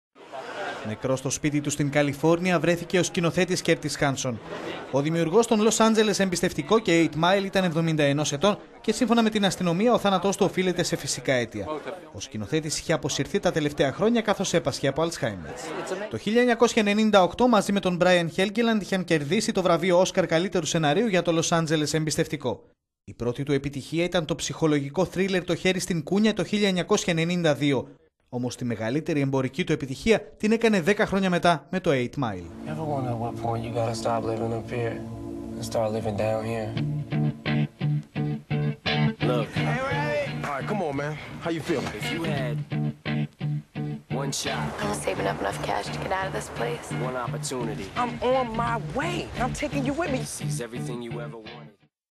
ΗΠΑ: Πέθανε ο σκηνοθέτης του «Λος Άντζελες: Εμπιστευτικό»
Νεκρός στο σπίτι του στην Καλιφόρνια βρέθηκε ο σκηνοθέτης Κέρτις Χάνσον.

Ο δημιουργός των "«Λος Άντζελες: Εμπιστευτικό»" και "«8 Mile»" ήταν 71 ετών και, σύμφωνα με την αστυνομία, ο θάνατός του οφείλεται σε φυσικά αίτια.

"I prefer stories about people who are trying to find better versions of themselves." CurtisHanson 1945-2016 Street Food Cinema (@stfoodcinema) September 21, 2016…